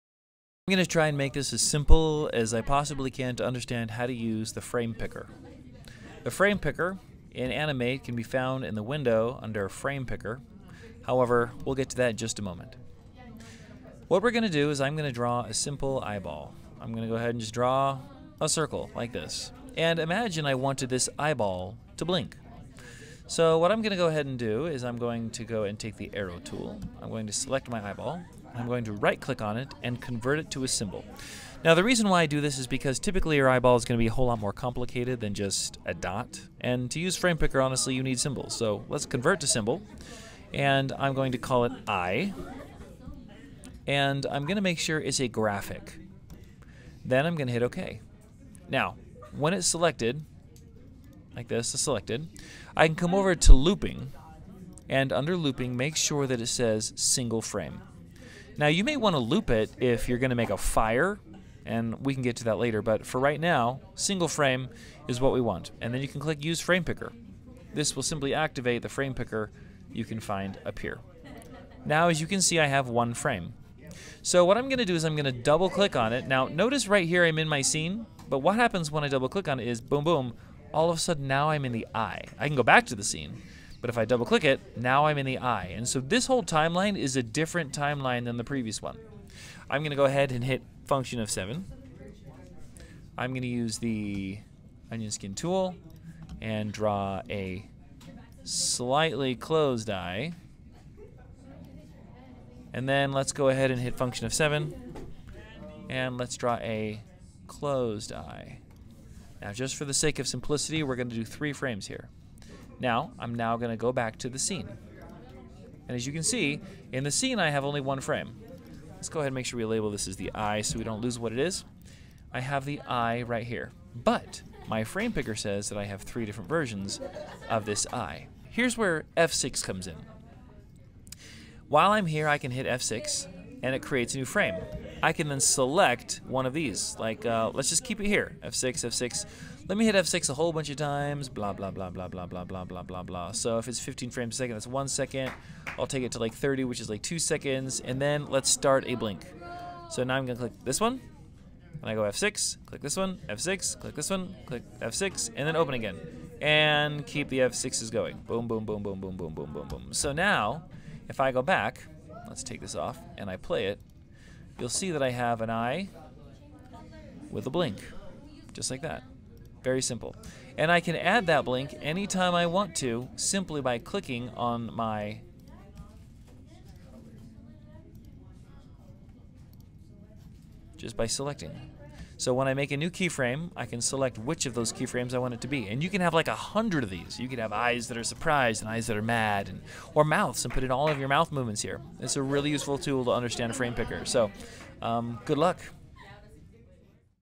Frame Picker
A simple tutorial on how to use Frame Picker in Adobe Animate.